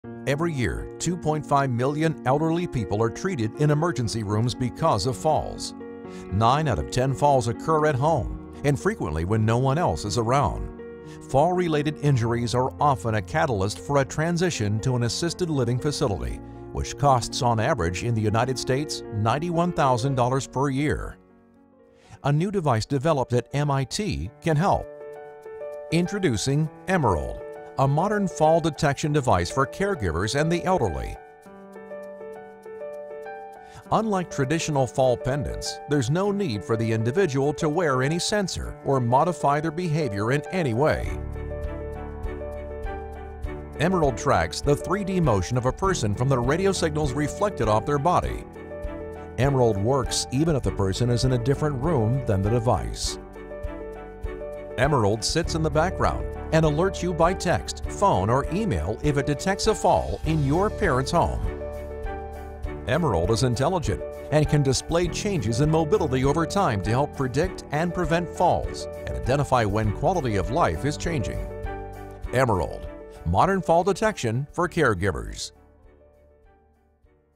MIT - emerald explainer video
MIT - emerald animated explainer video. Elderly people are often in danger when falling, to address this issue, a 25-year-old technology is still being used forcing people in risk to wear pendants. New innovative technology from MIT allows the home to be screened 24/7 by a device that will notify a 3rd party if a fall occurs at home.